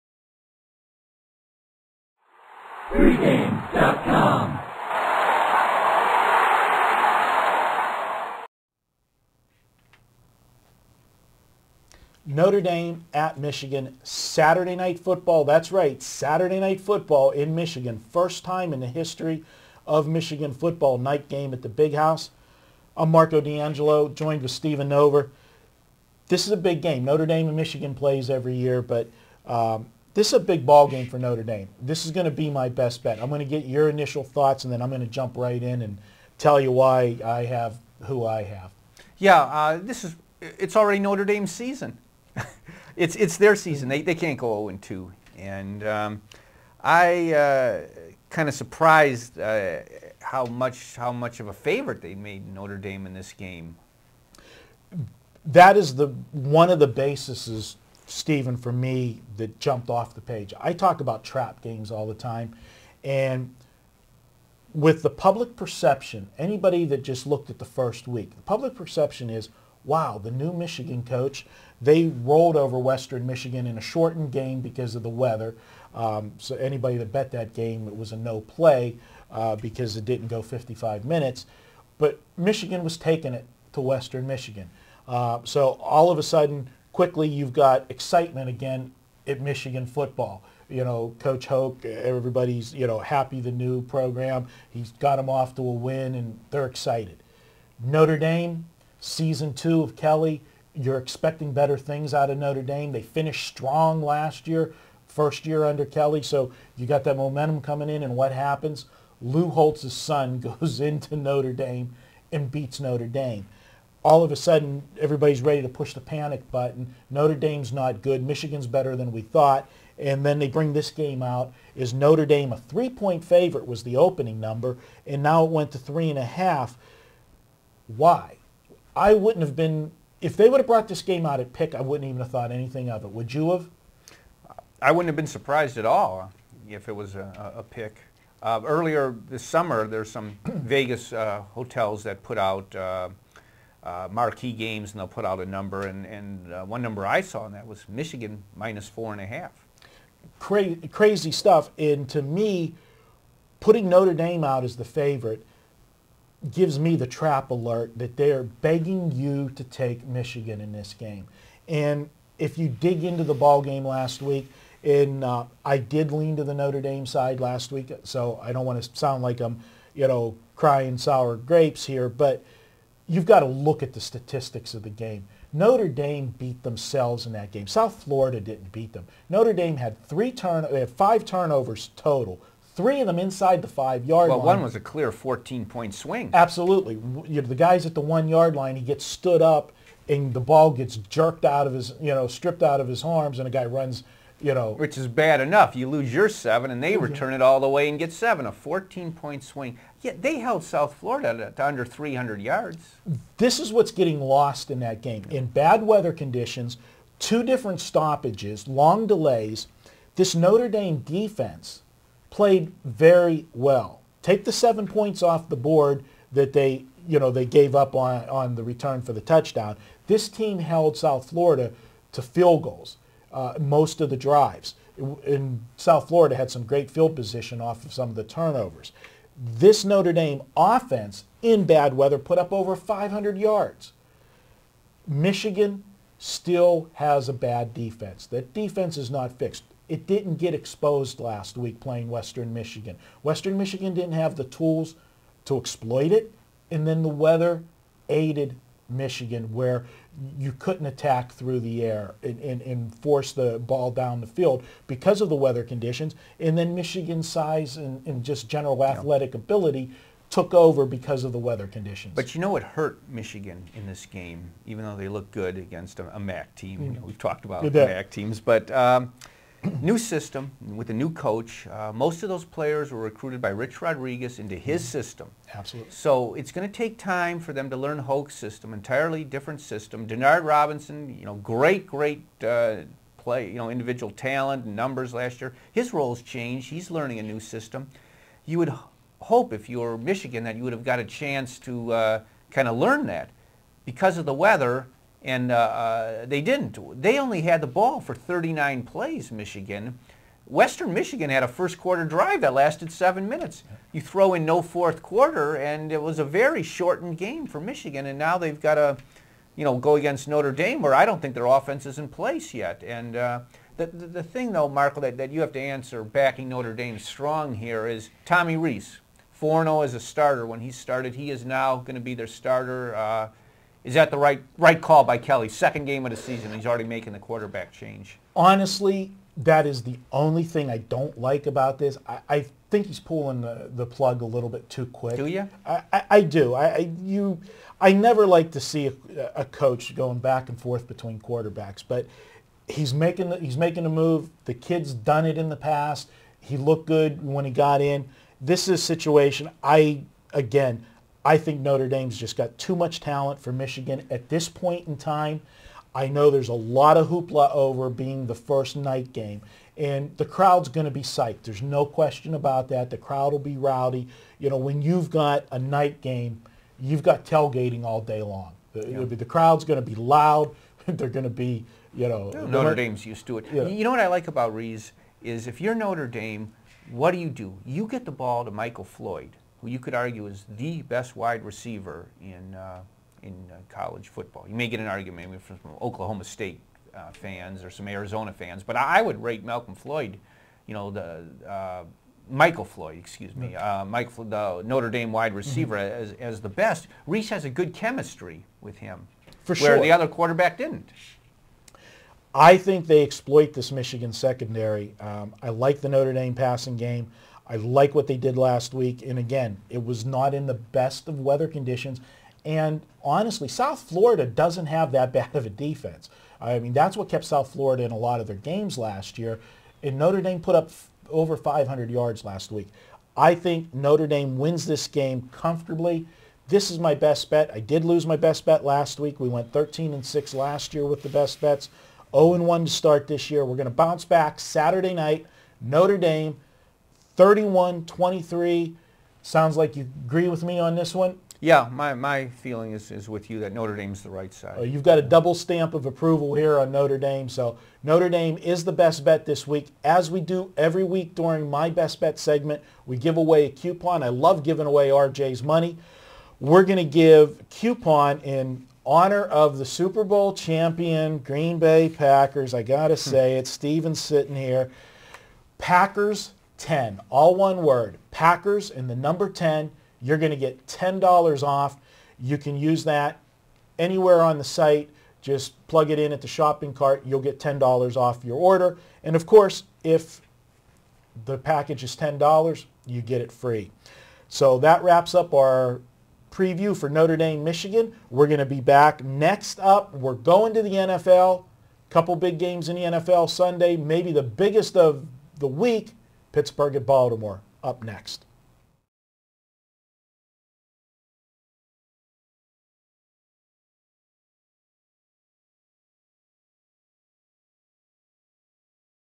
Free Picks: Notre Dame at Michigan - Including Best Bet from Marco D'Angelo
What NFL bettors need to know about Notre Dame at Michigan (Saturday, September 10th, 2011) from Pregame.com - Including a free best bet from Marco D'Angelo and analysis from Stephen Nover.